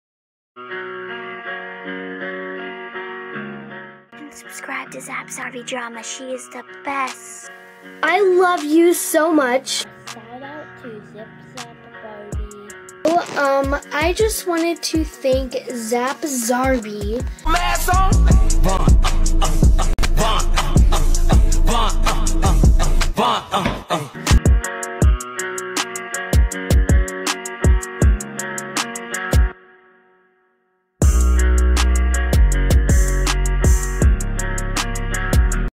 THANK YOU SO MUCH!!!♡♡♡
I just wanted to thank everyone who supports and follows me you guys are the best I love yall so much!!! I really am happy and just, have no words at all♡♡♡♡ thx for all the love and support yall, I love you guys so much!!!